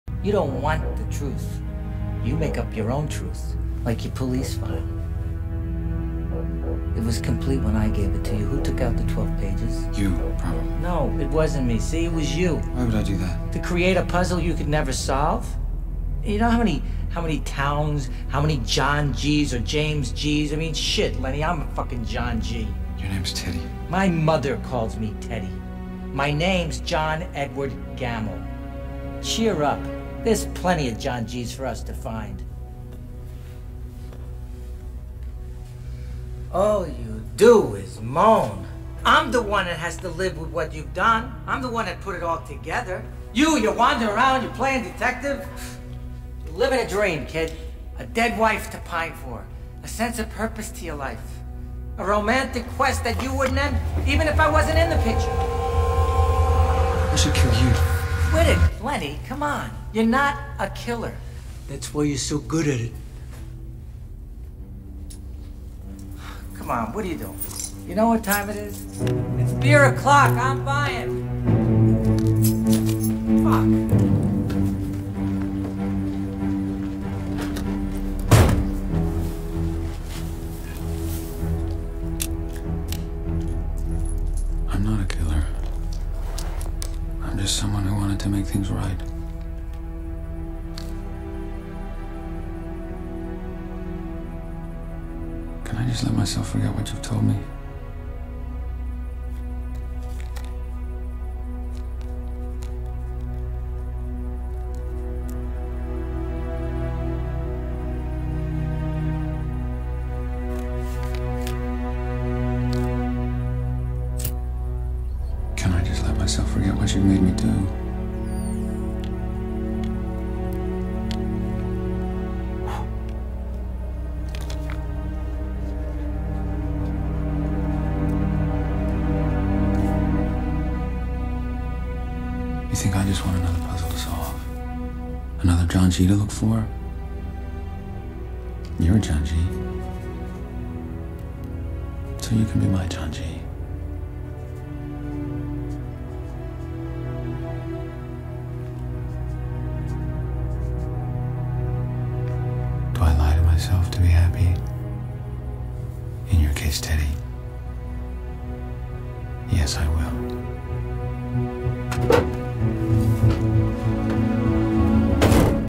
You don't want the truth. You make up your own truth.
"I'm not a killer. I'm just someone who wanted to make things right. Can't I just let myself forget what you've told me? Can't I just let myself forget what you've made me do. You think I just want another puzzle to solve? Another John G. to look for? You're John G. So you can be my John G... Will I lie to myself to be happy? In your case Teddy... yes I will."